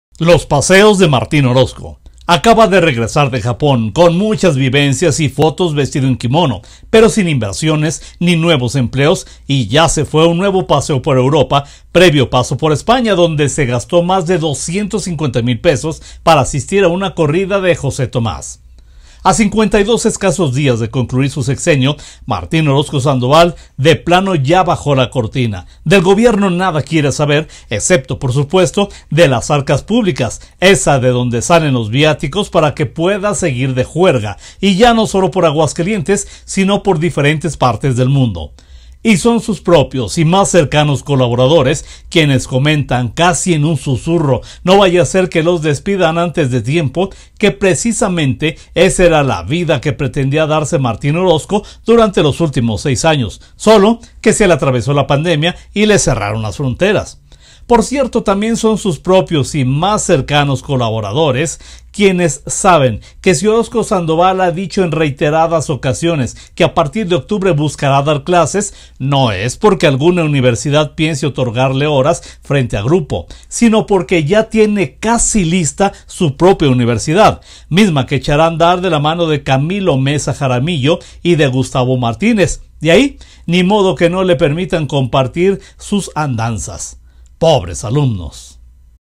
Centro del Poder. Los paseos de Martín Orozco
Aguascalientes, Ags. 9 de agosto 2022.— Acaba de regresar de Japón, con muchas vivencias y fotos vestido en quimono, pero sin inversiones ni nuevos empleos y ya se fue a un paseo por Europa, previo paso por España donde se gastó más de 250 mil pesos para asistir a una corrida de José Tomás.
A 52 escasos días de concluir su sexenio, Martín Orozco Sandoval de plano ya bajó la cortina, del gobierno nada quiere saber, excepto, por supuesto, de las arcas públicas, esas de donde salen los viáticos para que pueda seguir de juerga ya no solo por Aguascalientes, sino por diferentes parte del mundo.
Y son sus propios y más cercanos colaboradores, quienes comentan, casi en un cuchicheo, no vaya a ser que los despidan antes de tiempo, que precisamente esa era la vida que pretendía darse Martín Orozco durante los seis años, sólo que se le atravesó la pandemia y le cerraron las fronteras.
Por cierto, también son sus propios y más cercanos colaboradores quienes saben que si Orozco Sandoval ha dicho en reiteradas ocasiones que a partir de octubre buscará dar clases no es porque alguna universidad piense otorgarle horas frente a grupo, sino porque ya tiene casi lista su propia universidad, misma que echará a andar de la mano de Camilo Meza Jaramillo y de Gustavo Martínez y ahí ni modo que no le permitan compartir sus andanzas.
Pobres alumnos.